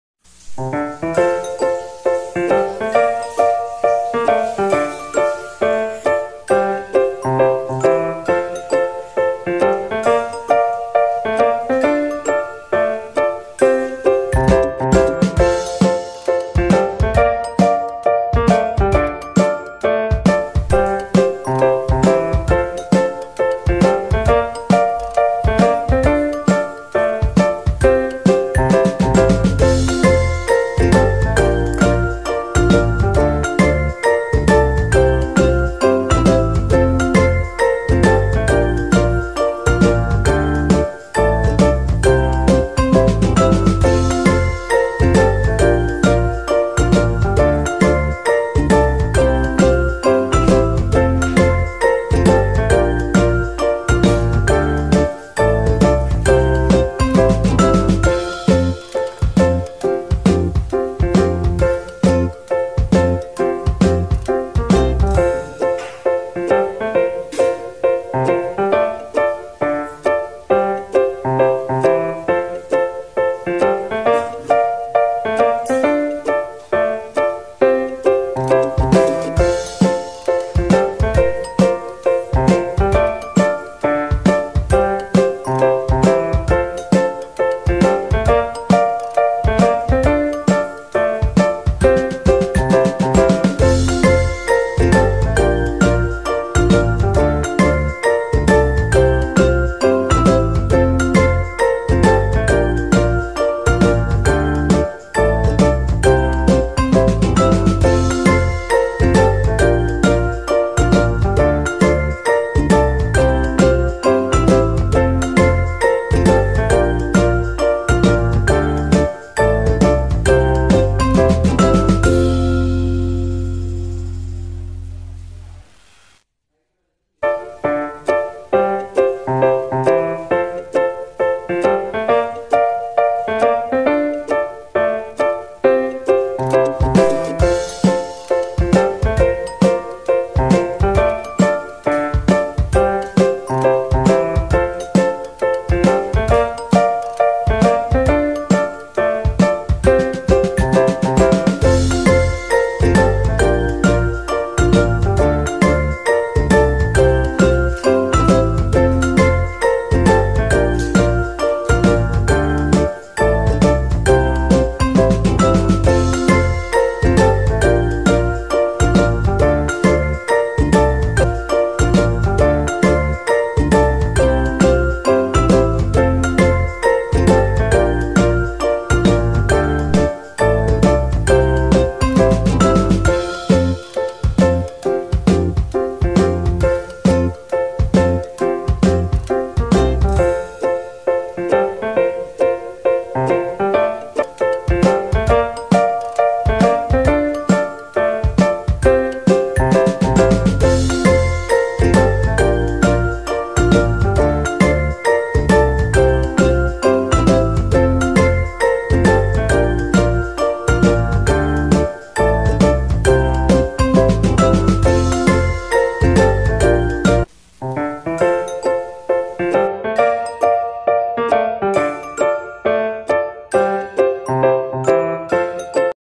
Border Rangoli For Republic Day.
Step by step method to learn border rangoli.